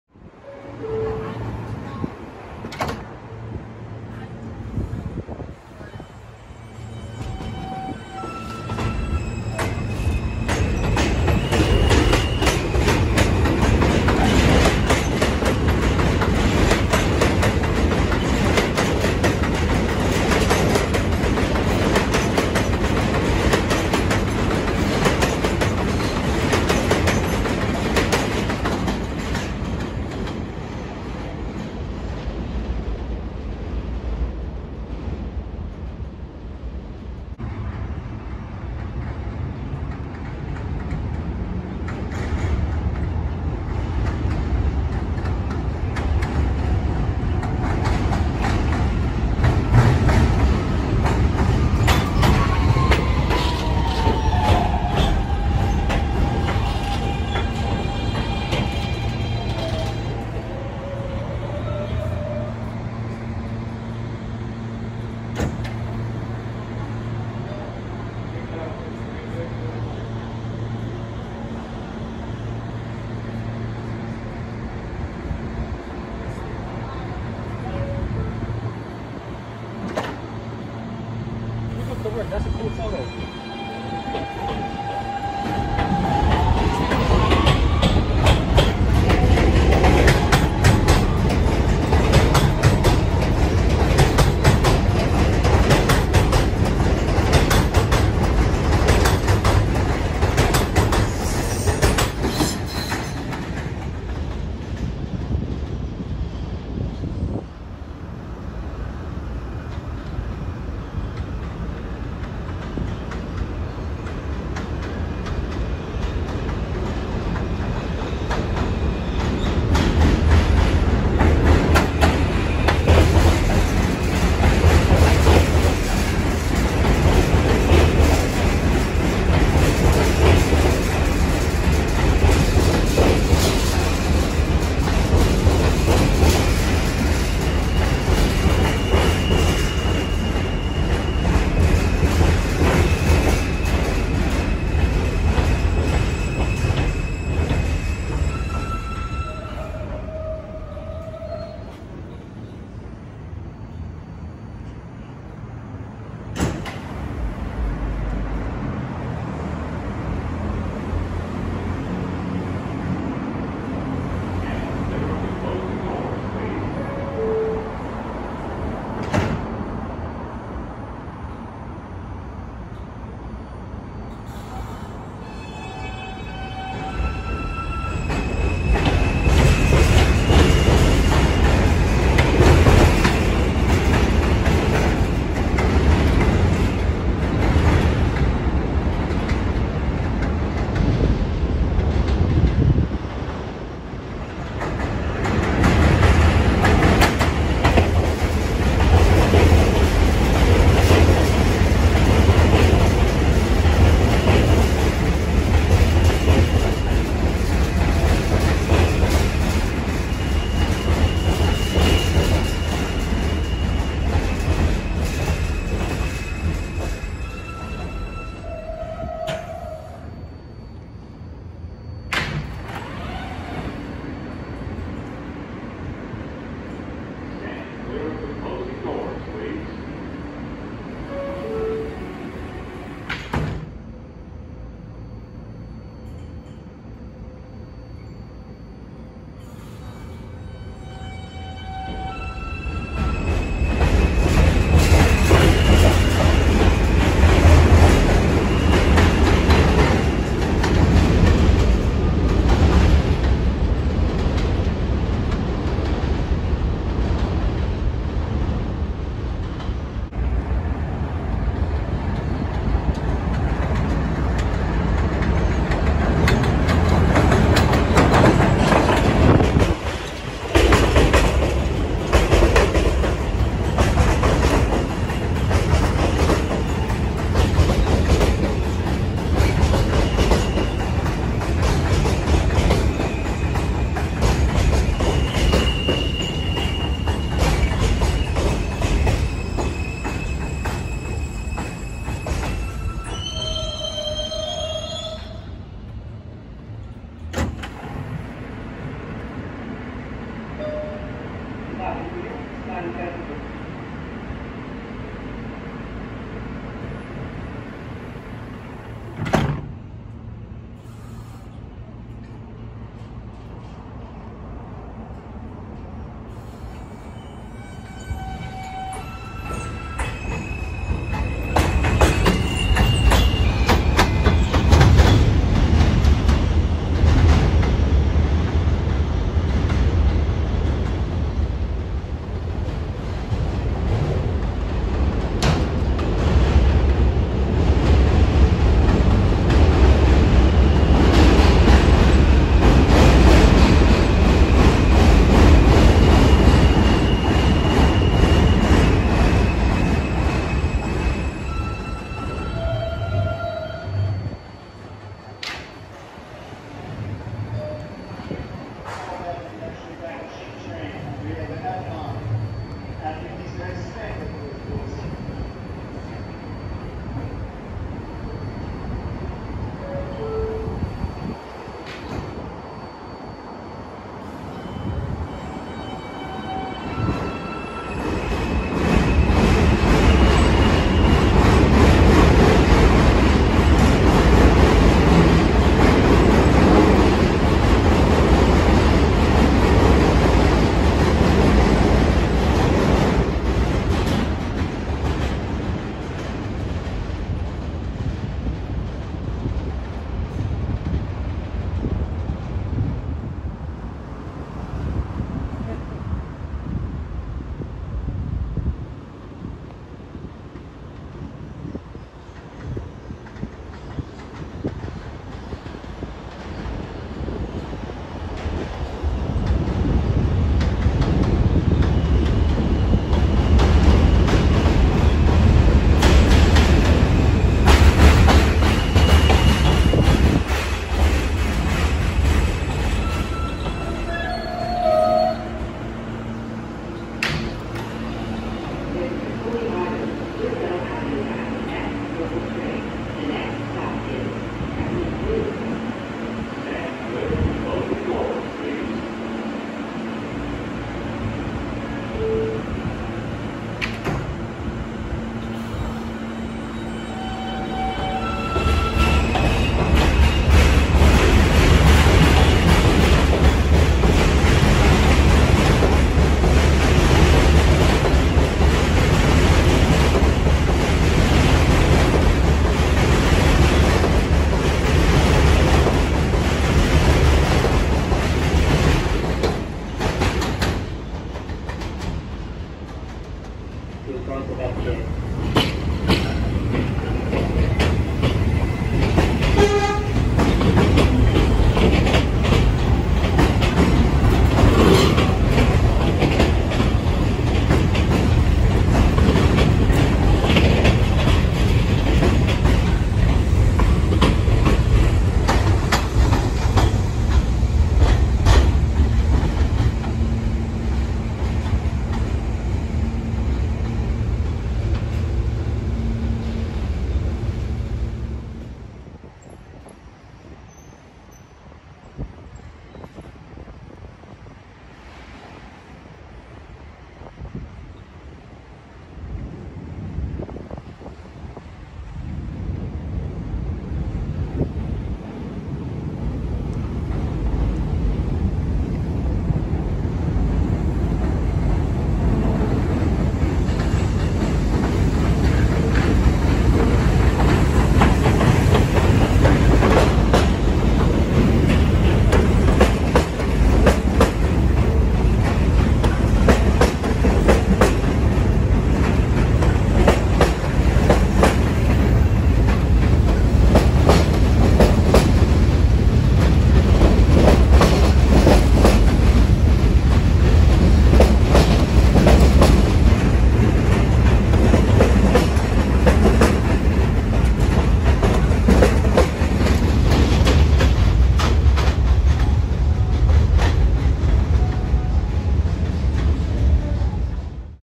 (G) Trains at Kings Highway (to 4 Av/9 St)
For this railfanning video, I was at Kings Highway on the BMT Culver Line, with R160A & R160B G Trains running to/from 4 Av-9 St and Coney Island-Stillwell Avenue to provide alternative service for the F while it was rerouted to Euclid Avenue.

ALTERNATE SERVICE
Aug 7 - 9  •  Aug 14 - 16, Sat 5 AM to Mon 5 AM
(G) Trains run between Court Square and Church Avenue and via the (F) to/from Coney Island-Stillwell Avenue.

TUNNEL REHABILITATION
Aug 6 - 7  •  Aug 13 - 14 , Fri 9:30 PM to Sat 5 AM
(F) trains are replaced by (E), (G), and free shuttle buses in Manhattan and Brooklyn.
(F) Trains run via the (E) in both directions between Roosevelt Avenue and Canal Street and via the (C) to/from Euclid Avenue, Brooklyn.

Timestamps:
0:00 | Introduction
0:37 | 4 Av-9 St bound G Trains
4:20 | Coney Island bound G Trains